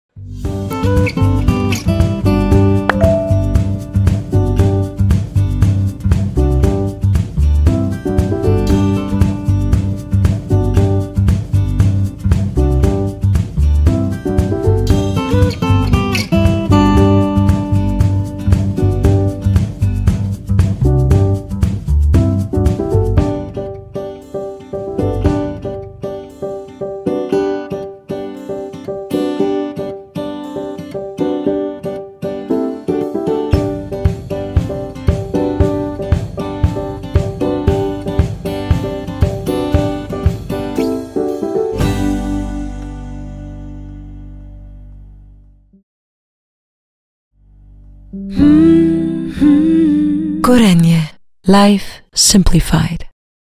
How it works ∙ Smart Connected Fridge Freezer with ConnectLife – AppliancesDashboard
Constant insight into your fridge & frezzer statuses.

Having control and constant information what is going on with your fridge & freezer is really a stress-free thing.

The new Smart Connected Fridge Freezers simplify our everyday life,
as they are here to meet our various needs.
Not only to keep our food safe and fresh, but they also help us
take more control over our food managing.